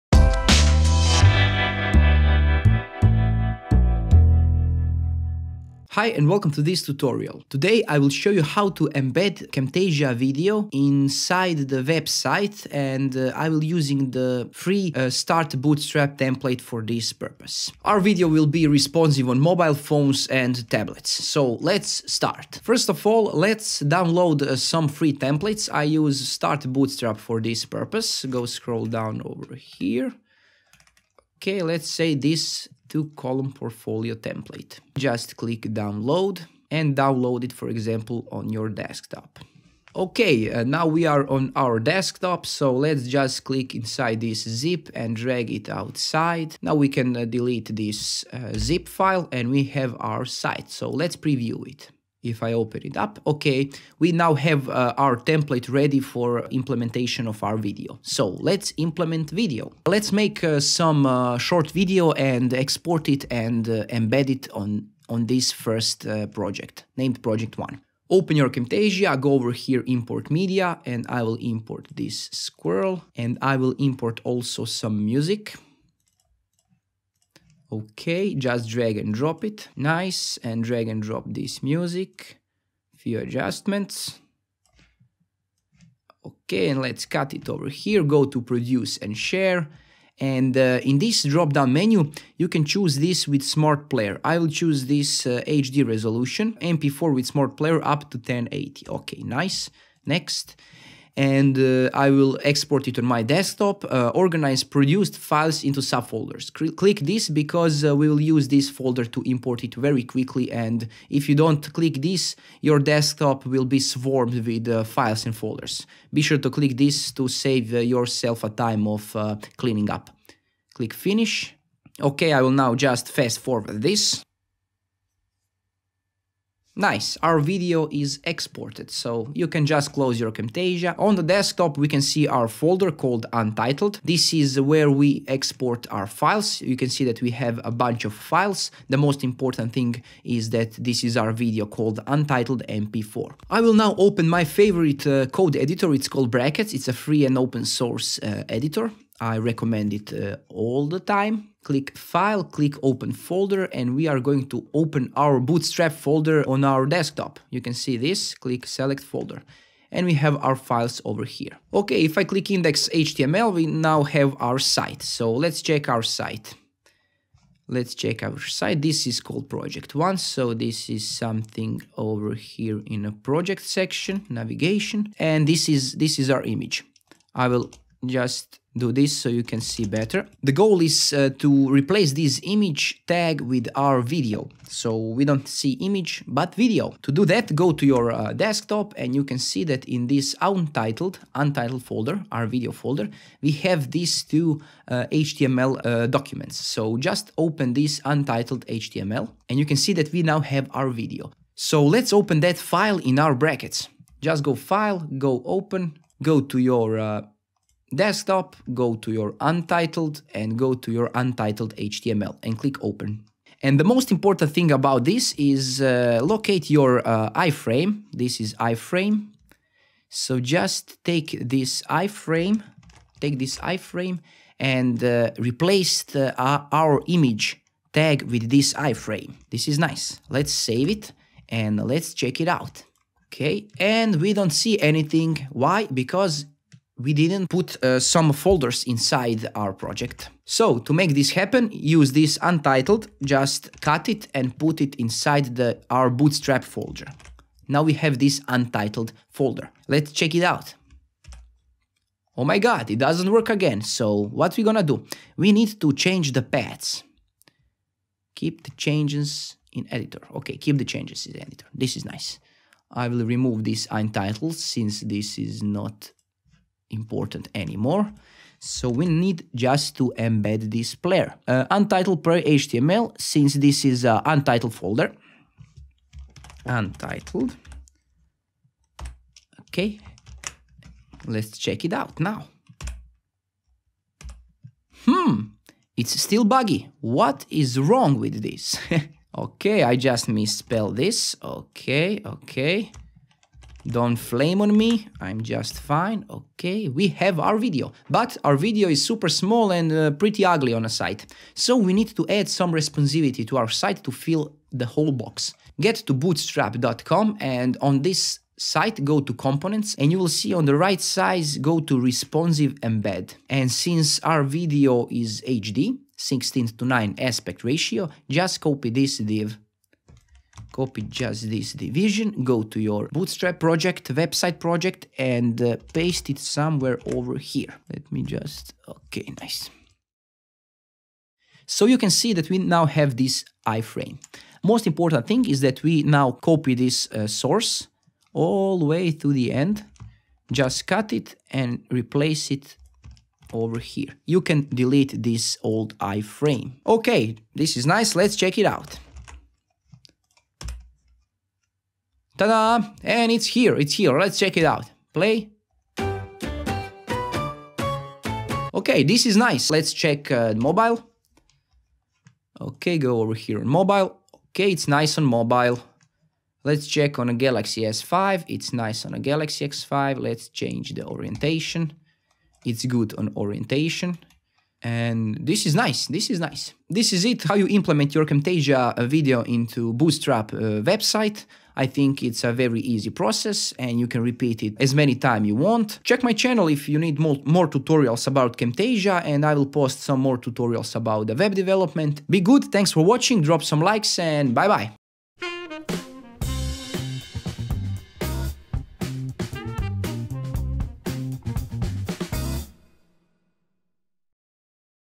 Insert Camtasia Video into Website
Hi and welcome to this tutorial. Today I will show you how to embed Camtasia video inside the website. And the I'm using the free start bootstrap template for this purpose.

Our video will be responsive on mobile phones and tablets so let's start. First of all, let's download some free templates I use start bootstrap for this purpose. Go scroll down over here. Ok, let's say these two columns portfolio template. Just click download and download it for example on your desktop. Ok, now we are on our desktop so let's just click on this zip and drag it outside. Now we can delete this zip file and we have our site. So let's preview it. If I open it up. Ok, we now have our template ready for implementation of our video. So let's implement video. Let's make some short video and export it and the embedded on on this first project named project one. Open Camtasia, go over, here import media and I will import this squirrel and I will import also some music ok. Just drag and drop it nicely. And drag and drop this music few adjustments ok. And let's cut it over here go to produce and share. And the in this drop-down menu you can choose this with the smart player. I will choose this HD resolution mp4 with the smart player up to 1080. Ok nice next. And I will export to my desktop to organize produced files into subfolders. Click this because we will use this folder to import it very quickly and if you don't click this your desktop will be swarmed with files and folders. Be sure to click this to save yourself a time of cleaning up. Click finish. Ok, I will now just fast forward this. Nice, our video is exported. So you can just close your Camtasia on the desktop we can see our folder called untitled. This is where we export our files you can see that we have a bunch of files, the most important thing is that this is our video called untitled mp4. I will now open my favorite code editor. It's called brackets it is a free and open source editor I recommend it all the time. Click file click open folder and we are going to open our bootstrap folder on our desktop you can see this click select folder and we have our files over here ok. If I click index HTML we now have our site so let's jack our site. This is called project 1 so this is something over here in a project section navigation and this is this is our image. I will just do this so you can see better. The goal is to replace this image tag with our video so we don't see the image but video. To do that go to your desktop and you can see that in this untitled folder, our video folder we have this two HTML documents. So just open this untitled HTML and you can see that we now have our video. So let's open that file in brackets. Just go file go open go to your desktop go to your untitled and go to your untitled HTML and click open. The most important thing about this is to locate your iframe this is iframe so just take this iframe take this iframe and replace our image tag with this iframe. This is nice. Let's save it and let's check it out. Ok, and we don't see anything. Why? Because we didn't put some folders inside our project so to make this happen to use this untitled just cut it and put it inside our bootstrap folder. Now we have this and untitled folder. Let's check it out .Oh my god it doesn't work again so what you going to do. We need to change the paths. Keep changes in the editor, ok, keep the changes in the editor. This is nice. I will remove this untitled since this is not important anymore. So we need just to embed this player. Untitled HTML since this is the untitled folder. Let's check it out now. It's still buggy what is wrong with this? Ok, I just misspell this ok don't flame me I'm just fine. Ok, we have our video, but our video is super small and pretty ugly on a site. So we need to add some responsivity to our site to fill the whole box. Get to bootstrap.Com and on this site go to components and you will see on the right side go to responsive embed and since our video is HD 16:9 aspect ratio, just copy this div, copy just this division, go to your bootstrap project website project and paste it somewhere over here. Let me just. Ok. Nice. So you can see that we now have this iframe. Most important thing is that we now copy this source all the way to the end, just cut it and replace it over here you can delete this old ifram.E ok this is nice let's check it out.